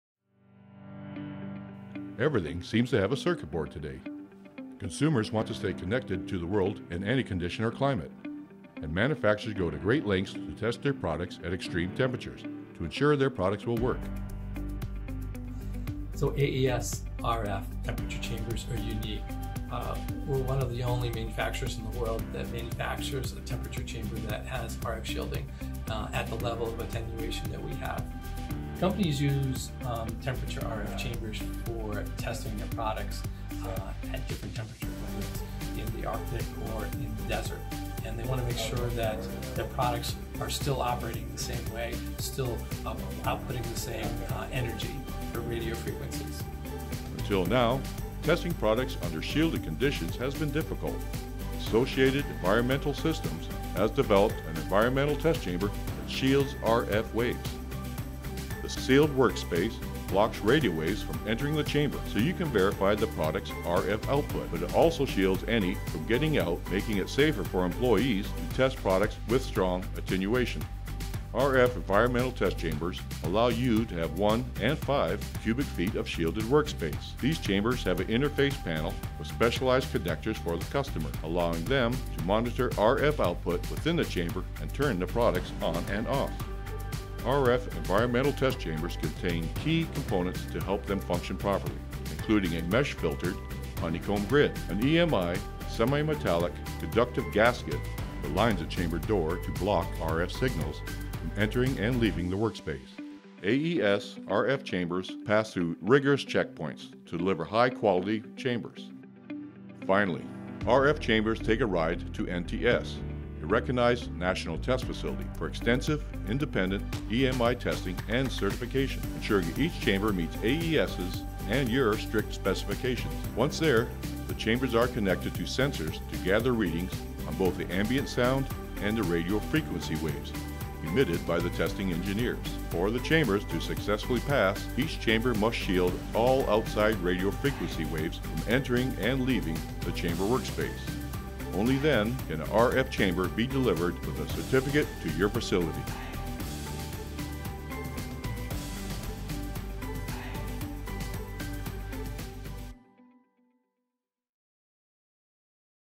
Radio Frequency Blocking Environmental Test Chamber
Associated Environmental Systems offers a temperature chamber that forms a conductive barrier to block radio frequency energy as you test circuit boards and other electronics. The FDR chamber offers the ability to test frequency generating devices within a shielded environment. The chamber also offers the ability to test devices inside that are shielded from the outside.